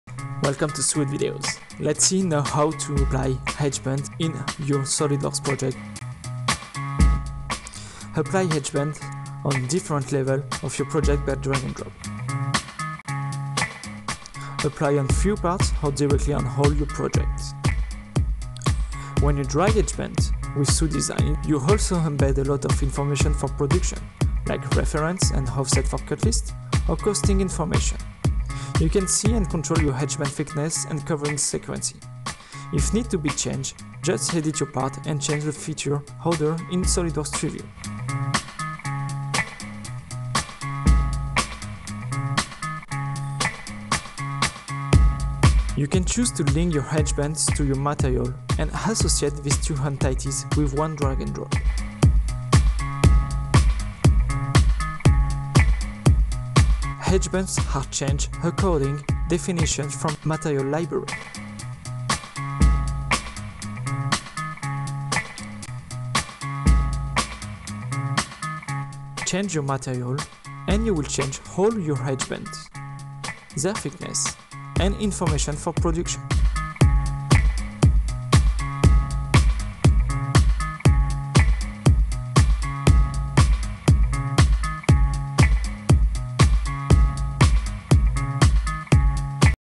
Edgeband
Complete your woodworking projects inside SOLIDWORKS with edgebanding management.